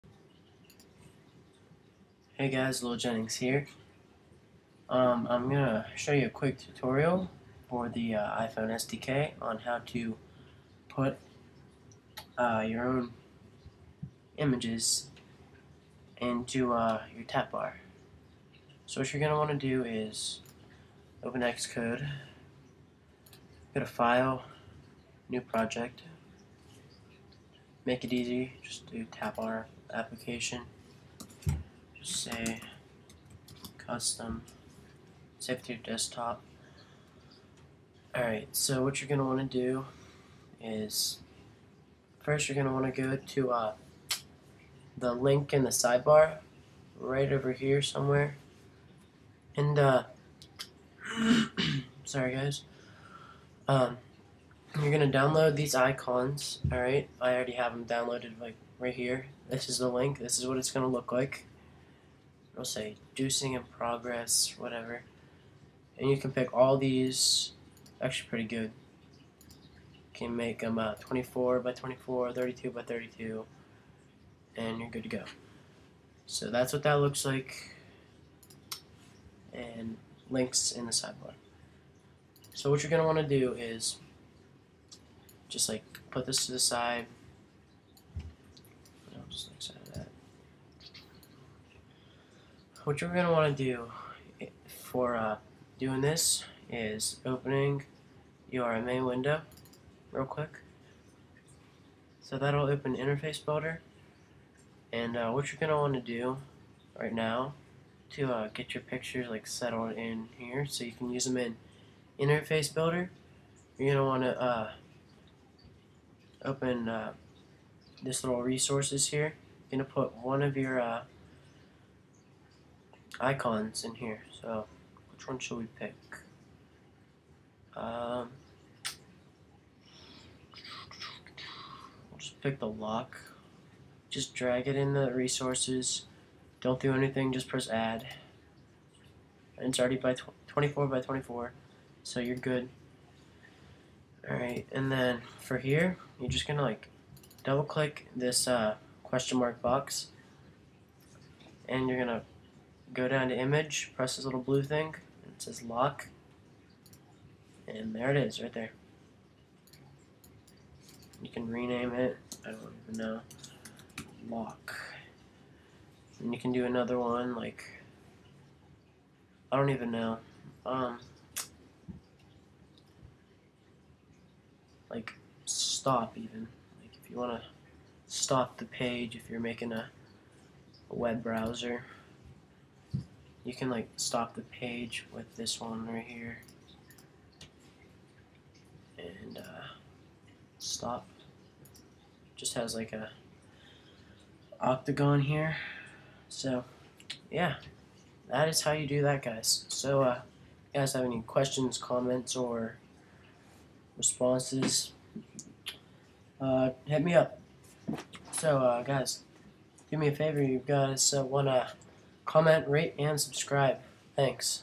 iPhone SDK Tutorial: How to other add images to your Tab/Tap Bar Application
In this video i show you how to use these simple icons to represent the buttons in your iPhone application.